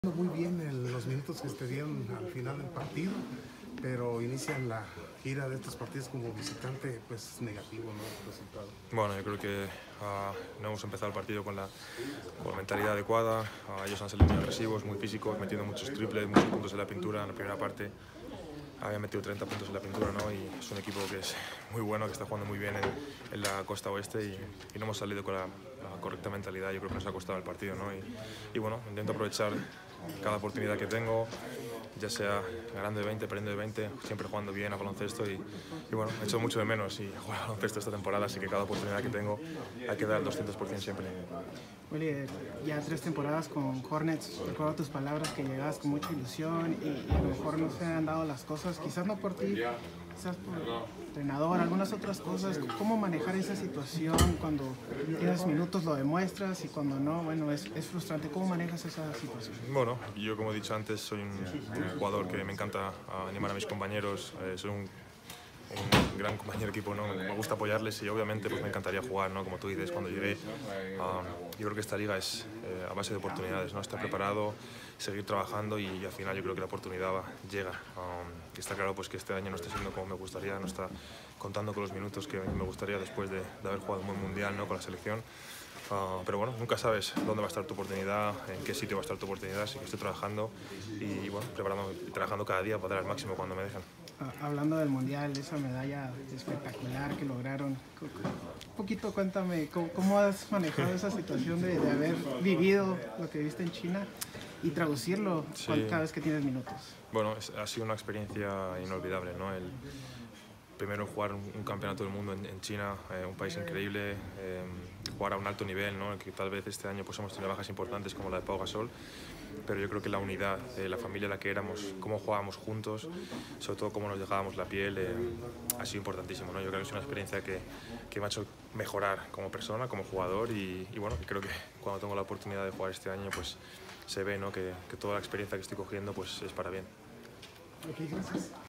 Entrevista con Willy Hernangomez Enero 10, 2020
Willy Hernangómez habla sobre los pocos minutos que esta recibiendo en la presente con su actual equipo los Hornets de Charlotte donde ha sido relegado a la tercera unidad, pocos minutos, pocas oportunidades por parte de su entrenador James Borrego.